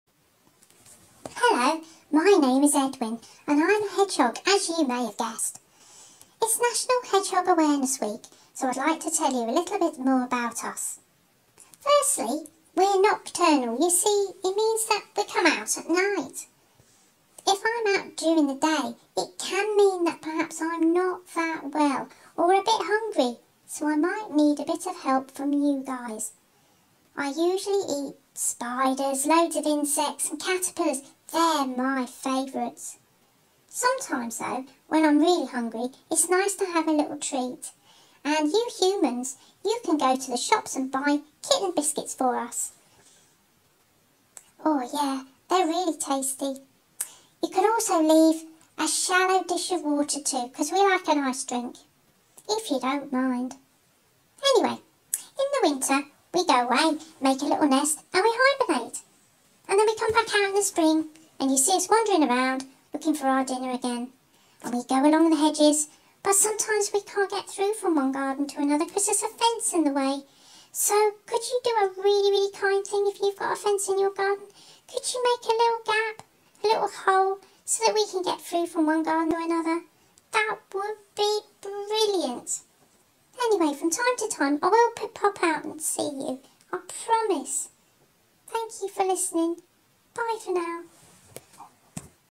Hedgehog Friendly Campus Scheme: Edwin the Hedgehog
Edwin has some words of wisdom for us all! Make sure to take heed of his words and help out your local hedgehog populations wherever possible.